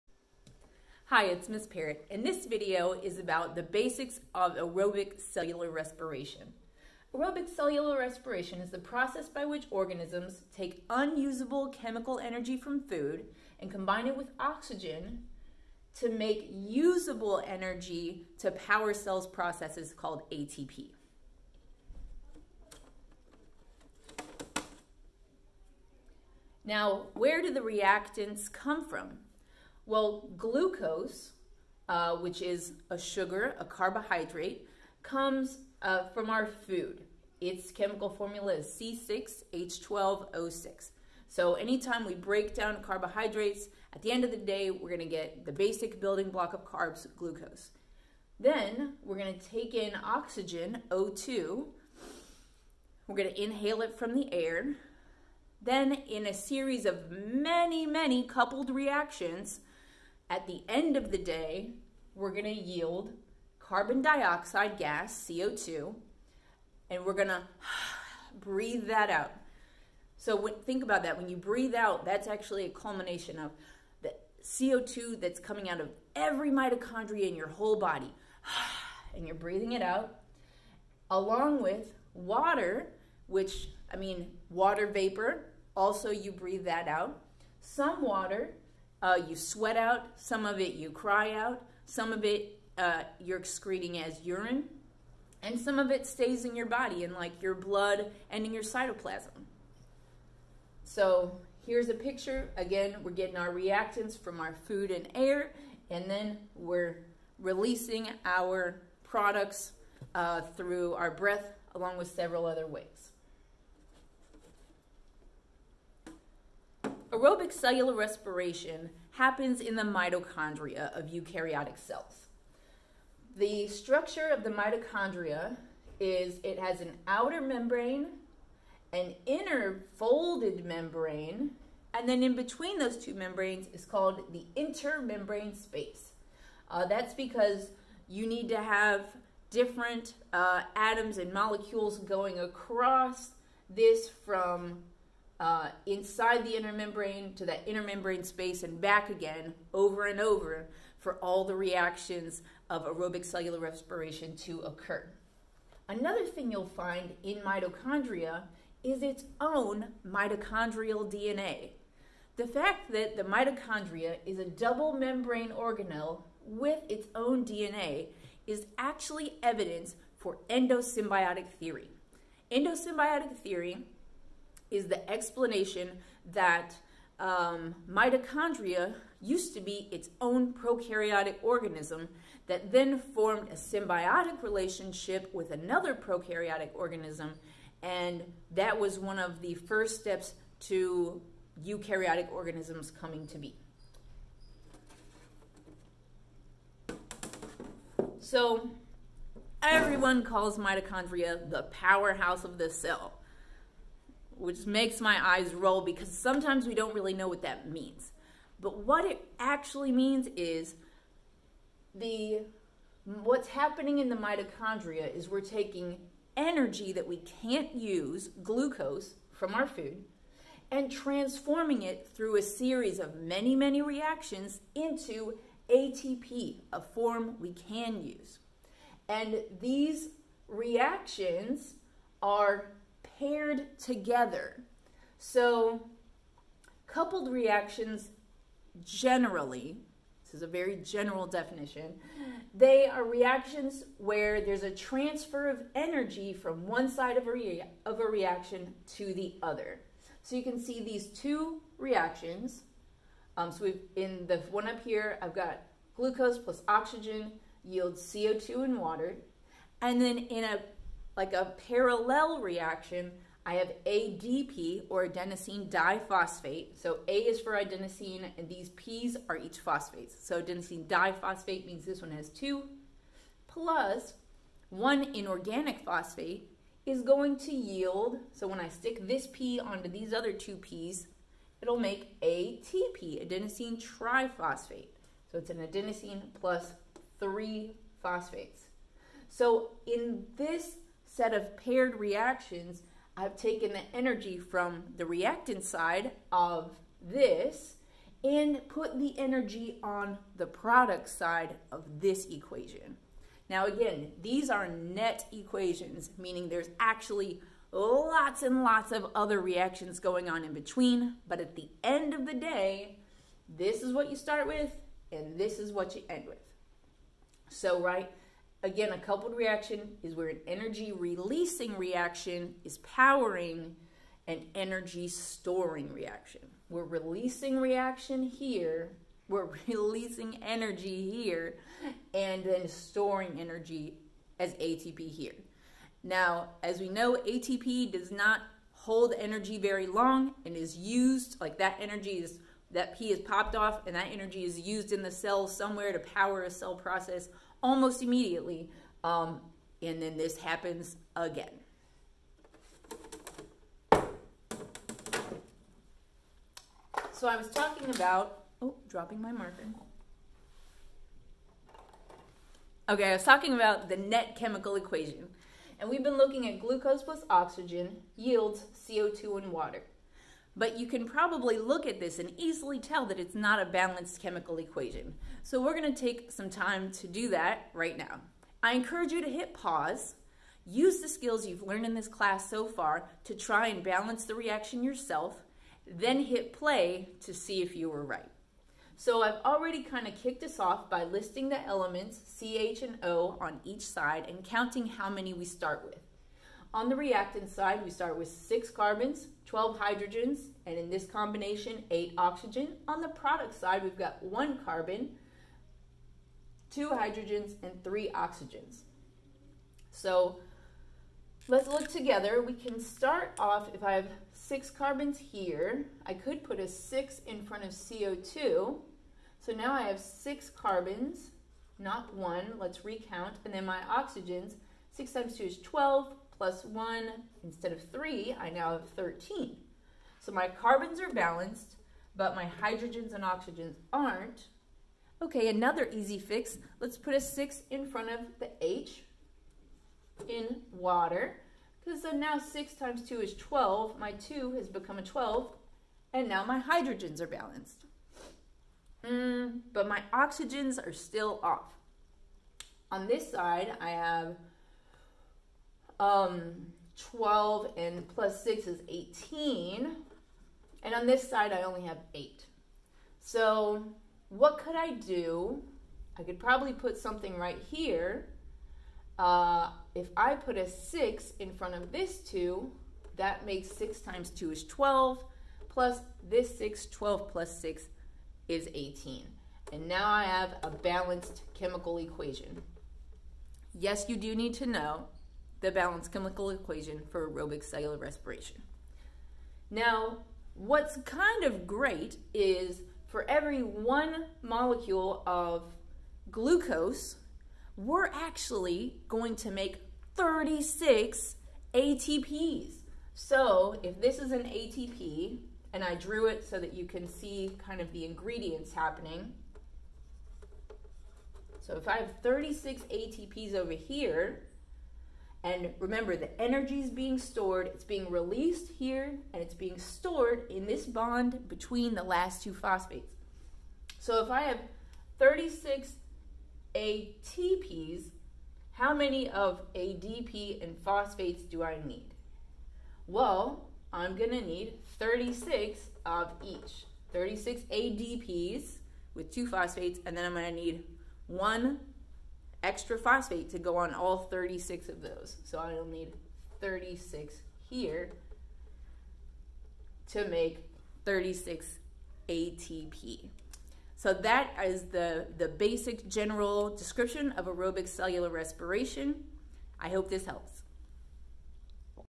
Aerobic Cellular Respiration Explained | Reactants, Products & ATP Production
Learn the basics of aerobic cellular respiration in this clear, step-by-step biology lesson. Ms. Parrott breaks down how organisms convert glucose and oxygen into ATP — the energy currency of the cell — while releasing carbon dioxide and water.

In this video, you’ll learn:
What aerobic cellular respiration is and why it’s essential for life
The role of glucose (C₆H₁₂O₆) and oxygen (O₂) as reactants
How mitochondria power the cell (and what “powerhouse” really means)
The balanced chemical equation for cellular respiration
How coupled reactions transform unusable energy into ATP
The number of ATP molecules produced per glucose molecule
How this process supports all cell activities

Perfect for biology students, AP Biology review, and anyone curious about cellular energy.

Timestamps:
0:00 Introduction to aerobic cellular respiration
0:29 Where the reactants come from
1:56 Mitochondria structure and function
3:18 What “powerhouse of the cell” really means
5:56 Understanding coupled reactions
6:02 Balancing the chemical equation
8:09 ATP yield per glucose molecule